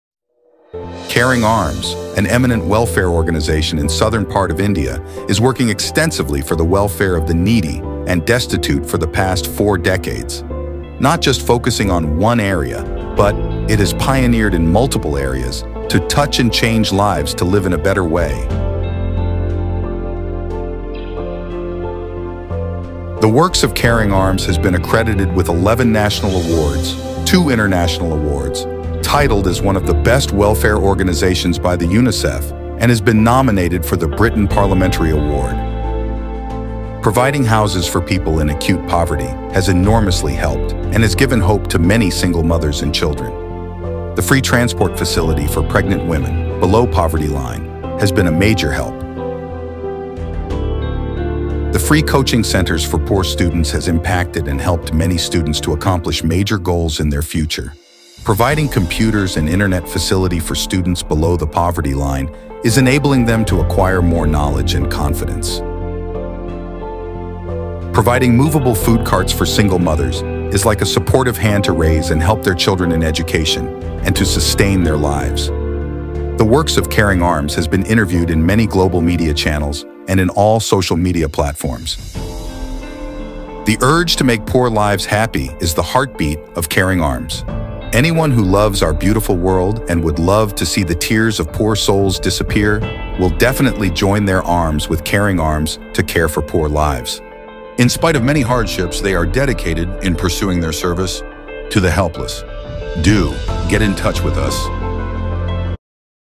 caringarms an ngo that reaches the unreached.7 national & international awards,Award by Unicef
The works of Caring Arms has been accredited with 11 national awards, 2 international awards, Titled as one the best Welfare organisations by the Unicef and has been nominated for the Britain Parliamentary award.
Providing Houses for people in acute poverty has enormously help and has given hope to many Single Mothers and Children,
The Free Transport facility for pregnant women,below poverty line has been a major help.
The free Coaching centres for poor students has impacted many students to accomplish major goals in their future.
Providing Computers and Internet facility for students below the poverty line is enabling them to acquire more knowledge and confidence.
Providing movable food carts for Single Mothers is like a supportive hand to raise and help children in their education..
The urge to make poor lives happy is the heart beat of Caring Arms